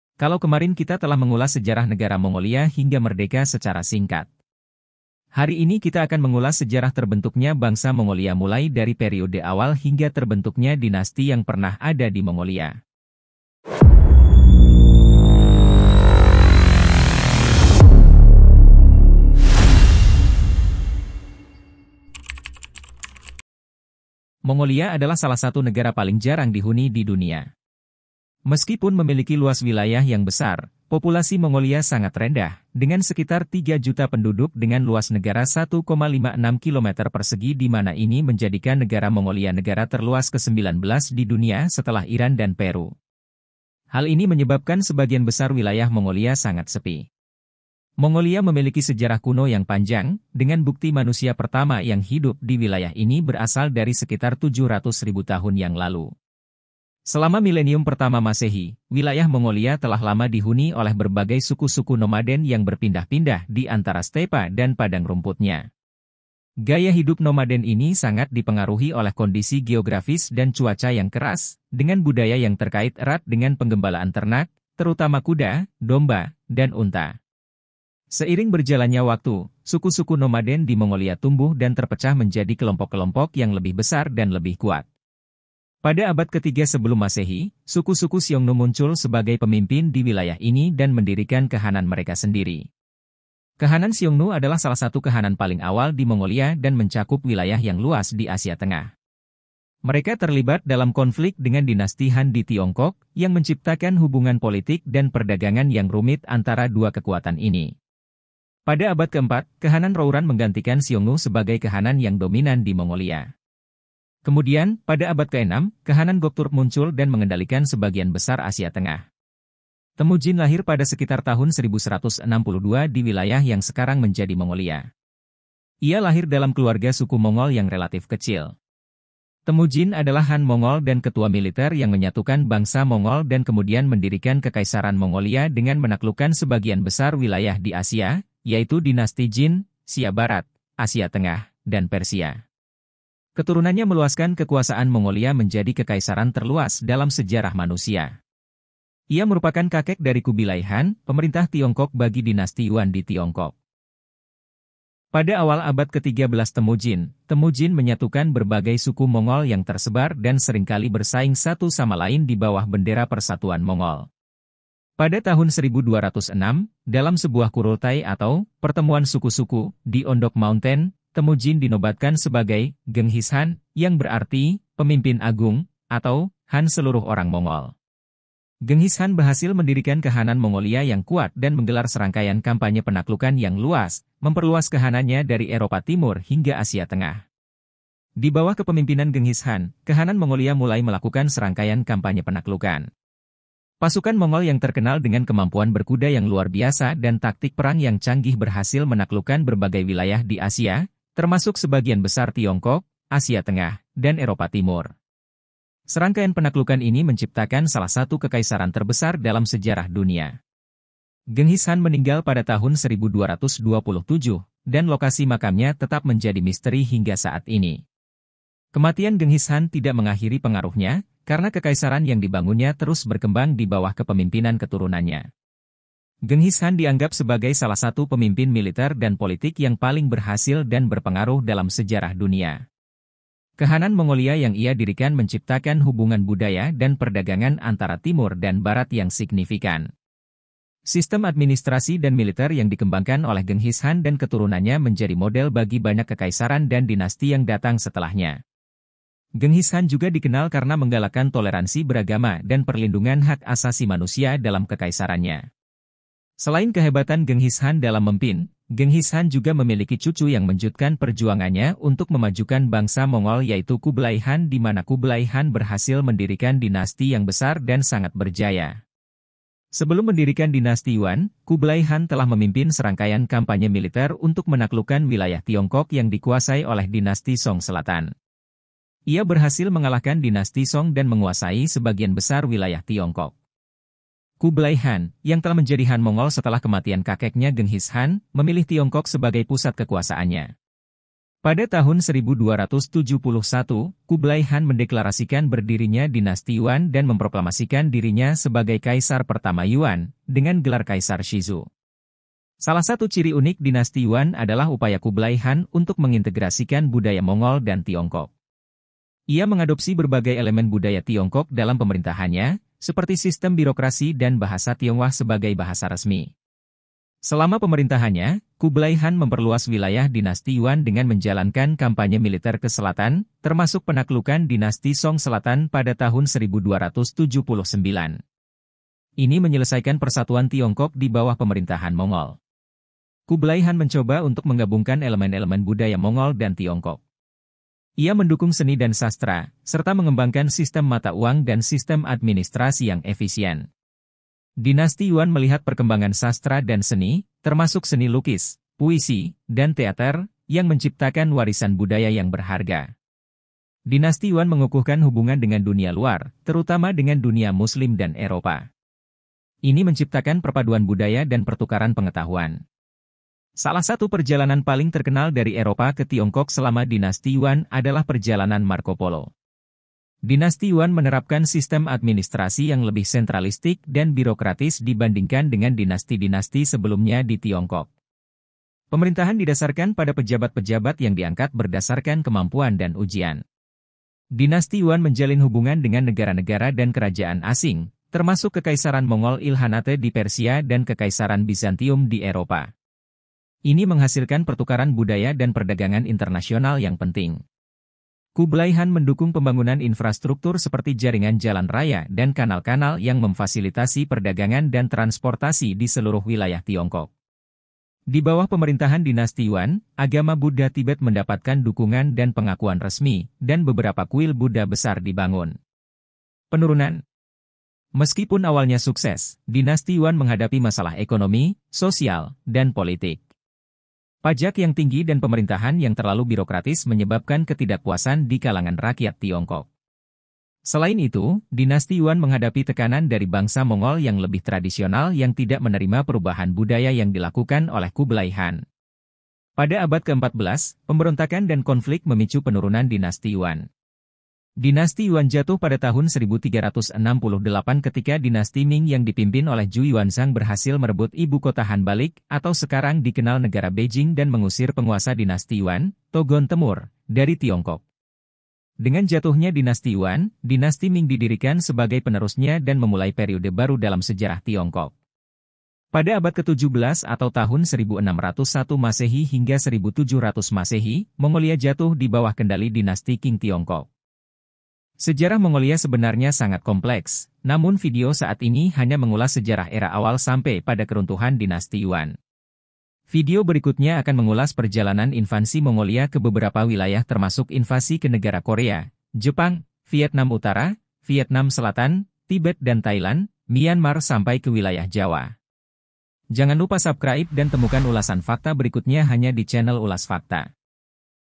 𝐄𝐑𝐀 𝐊𝐄𝐉𝐀𝐘𝐀𝐀𝐍 𝐌𝐎𝐍𝐆𝐎𝐋𝐈𝐀: 𝐌𝐄𝐌𝐈𝐋𝐈𝐊𝐈 𝐏𝐀𝐒𝐔𝐊𝐀𝐍 𝐓𝐄𝐑𝐊𝐔𝐀𝐓 𝐃𝐈𝐏𝐈𝐌𝐏𝐈𝐍 𝐉𝐄𝐍𝐆𝐇𝐈𝐒 𝐊𝐇𝐀𝐍 & 𝐂𝐔𝐂𝐔𝐍𝐘𝐀 𝐁𝐄𝐑𝐊𝐔𝐀𝐒𝐀 𝐃𝐈 𝐂𝐇𝐈𝐍𝐀
Mongolia memiliki sejarah kuno yang panjang, dengan bukti manusia pertama yang hidup di wilayah ini berasal dari sekitar 700.000 tahun yang lalu. Selama milenium pertama Masehi, Wilayah Mongolia telah lama dihuni oleh berbagai suku-suku nomaden yang berpindah-pindah di antara stepa dan padang rumputnya. Gaya hidup nomaden ini sangat dipengaruhi oleh kondisi geografis dan cuaca yang keras, dengan budaya yang terkait erat dengan penggembalaan ternak, terutama kuda, domba, dan unta. Seiring berjalannya waktu, suku-suku nomaden di Mongolia tumbuh dan terpecah menjadi kelompok-kelompok yang lebih besar dan lebih kuat.

Sejarah Mongolia sebenarnya sangat kompleks namun video saat ini hanya mengulas Sejarah era awal sampe pada keruntuhan dinasti Yuan. Video berikutnya akan mengulas perjalanan invansi Mongolia ke beberapa wilayah termasuk invasi ke negara korea, jepang, Vietnam utara, Vietnam Selatan, Tibet dan dan Thailand, Myanmar sampa ke wilayah jawa.